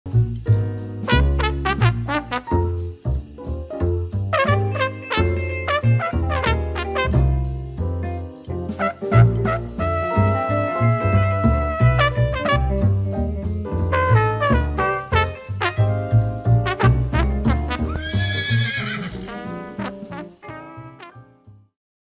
A reminder by Melvin the donkey and Isadora the pig - Easy Horse Care Rescue Centre
Just a little reminder, by Melvin the donkey and Isadora the pig!